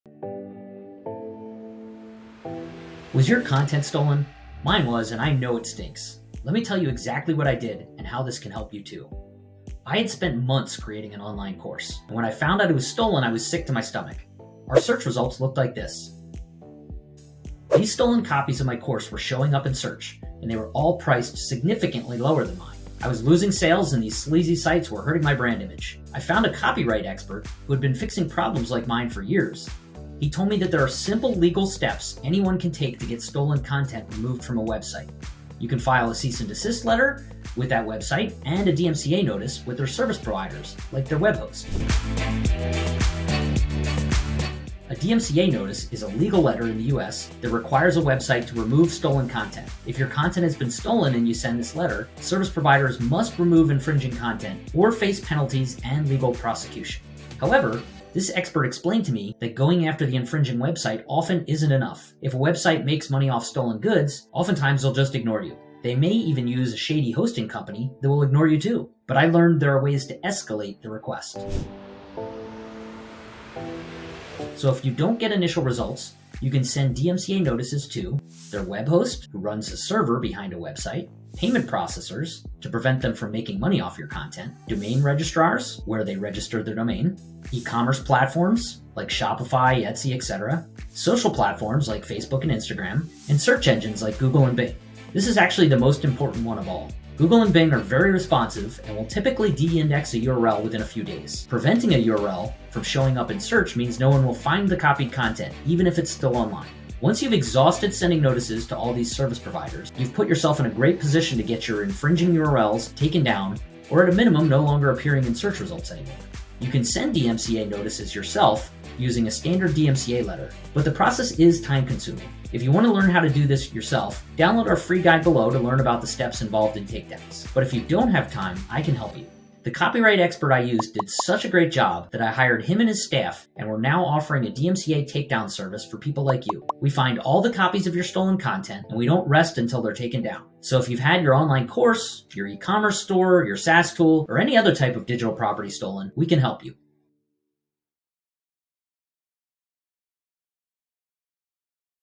How to Reclaim YOUR Stolen Content
Learn more about our service or sign up for our free guide on DMCA takedowns

After creating my course for months, I discovered it had been stolen and sold on many websites at a lower price, hurting my sales and brand.

Feeling frustrated, I needed help. If you're in a similar situation, I get it.

Luckily, I found an expert who knew what to do. He developed a process to find these sites and issued DMCA takedown notices to their hosts, payment processors, Google, and Bing.

His work was so effective that we joined forces to help others. Now, we assist creators, e-commerce, and anyone dealing with stolen digital content.